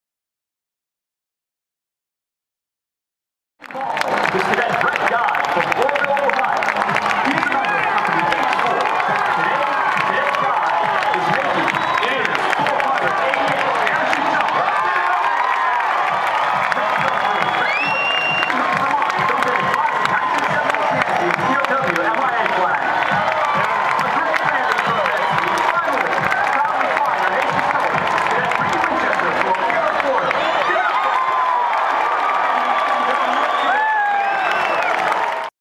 Army v. Tulane parachuting
home game - parachuting the game ball in!
6 October 2007